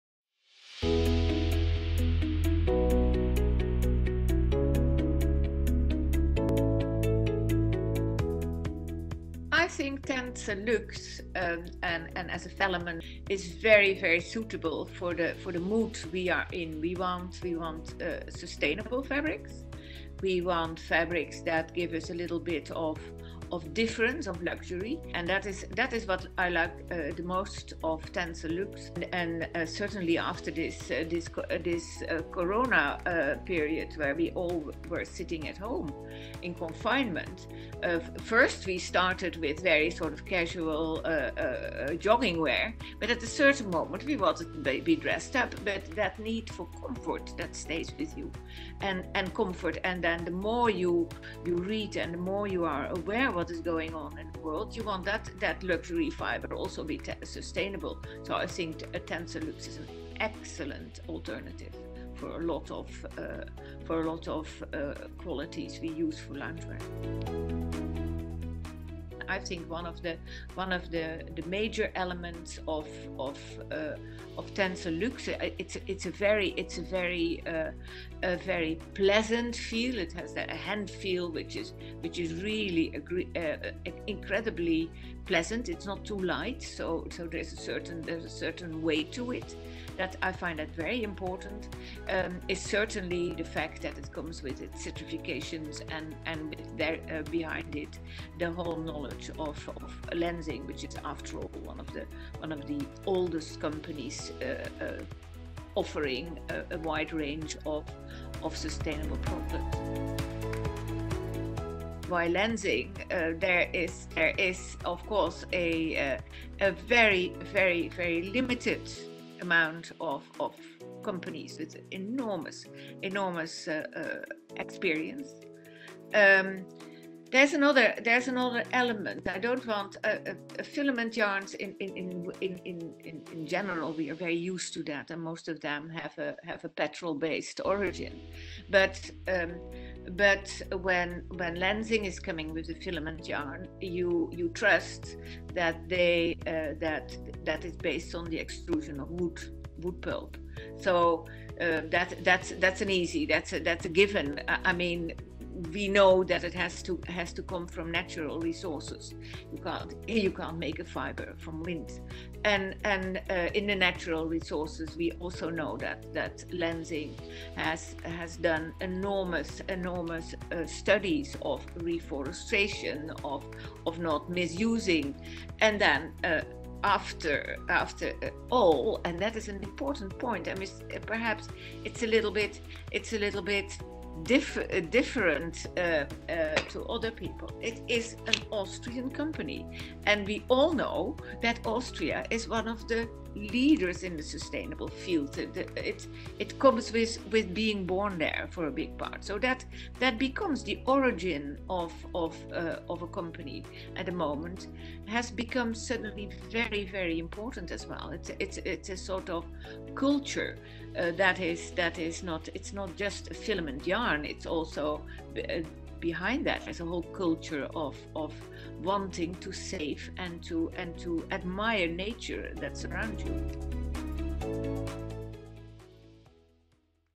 TENCEL™ Luxe x Jos Berry Interview
As working from home starts to become a norm for most of us, comfort is no longer the only quality we look for in our home wears.

Curious to know how the lockdown has shaped our fashion choices? Take a look at the below interview with Jos Berry, Founder and CEO of Concepts Paris, who just launched a new loungewear collection using TENCEL ™ Luxe branded filament yarns to learn more!